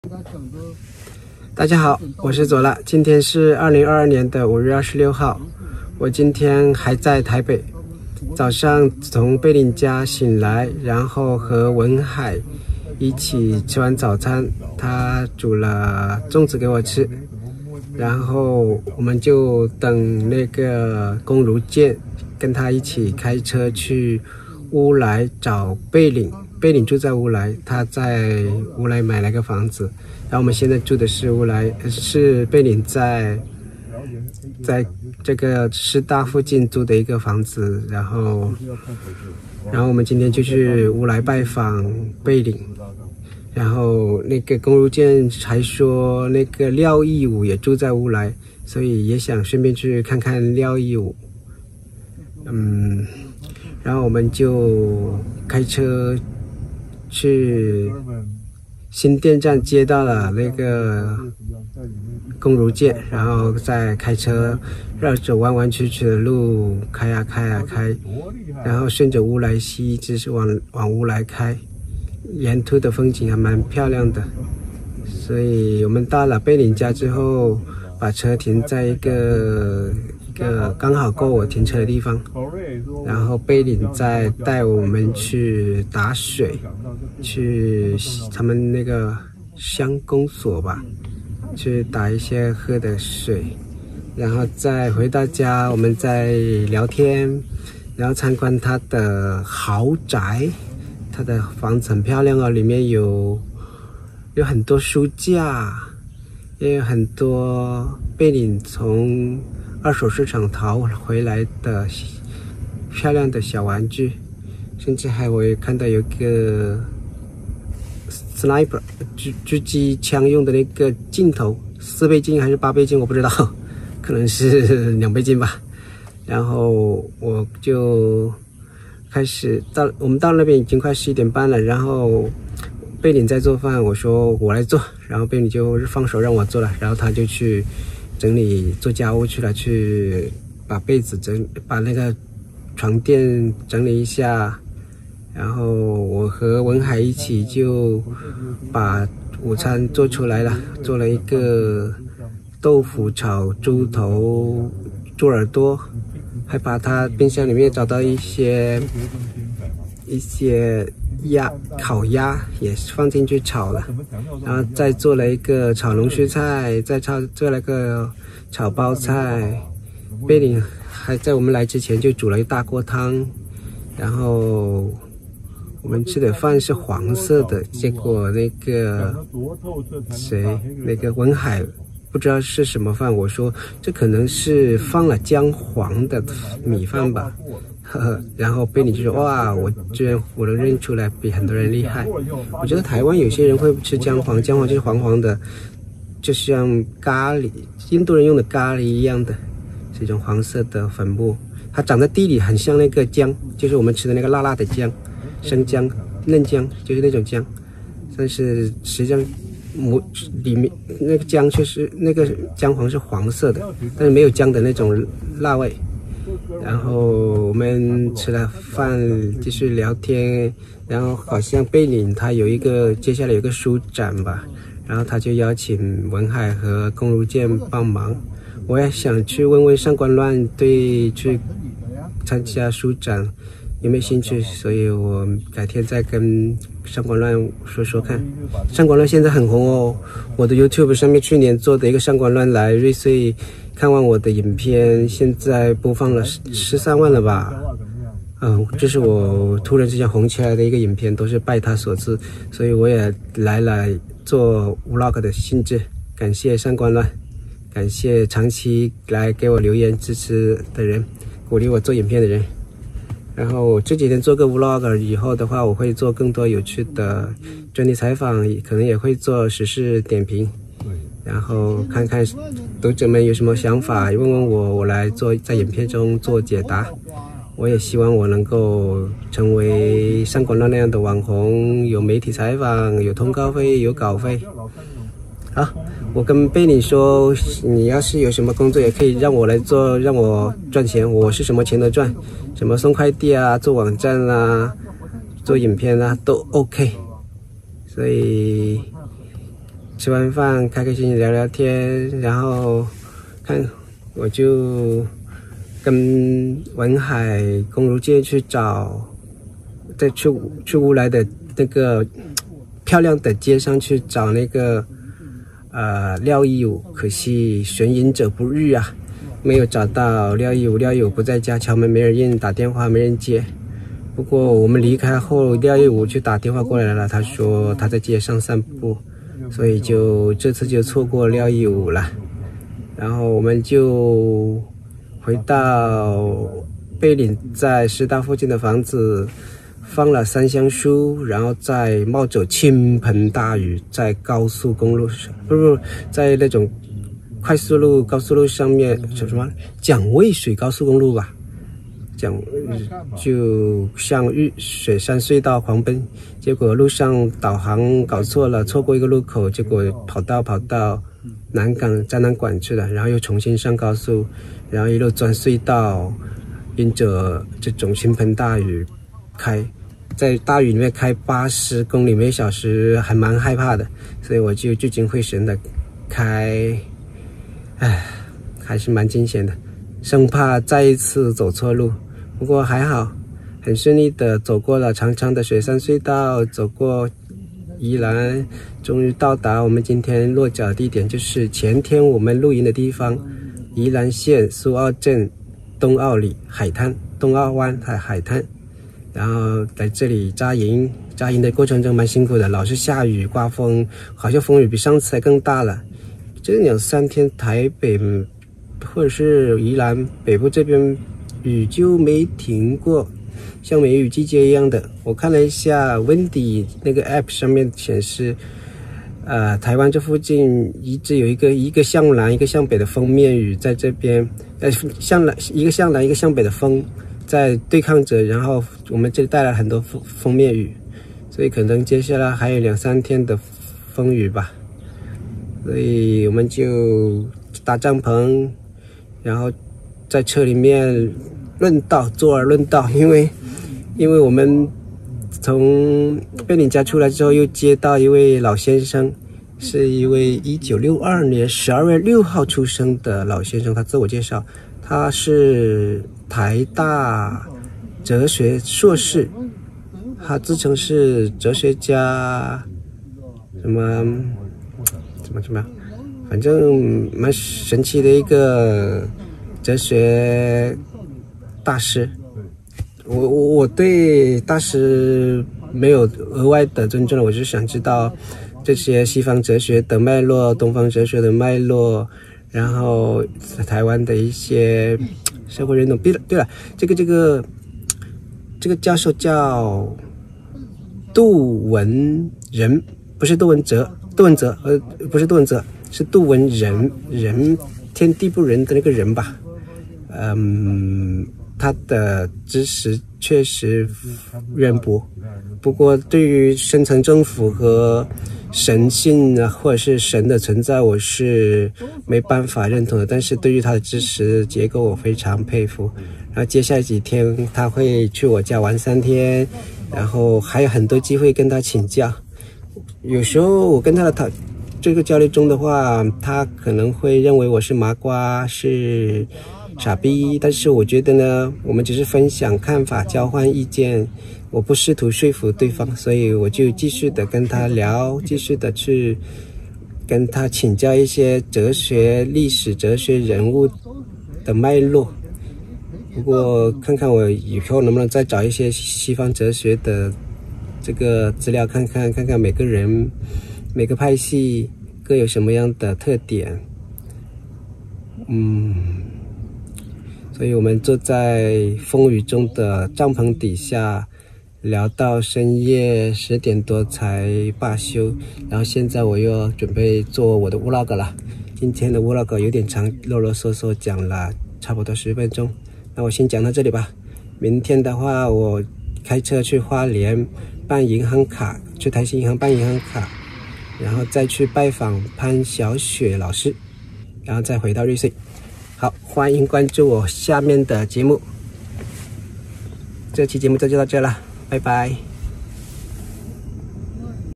(VLOG有字幕)早上佐拉和聞海、龔如劍前往烏來拜訪貝嶺，傍晚再次野營於宜蘭東澳海灘
佐拉前住烏來體驗山中豪宅的無敵美景後，再驅車走雪山隧道，平安抵達宜蘭的東澳海灘再度野營，得遇「道人」杜文仁談今論古。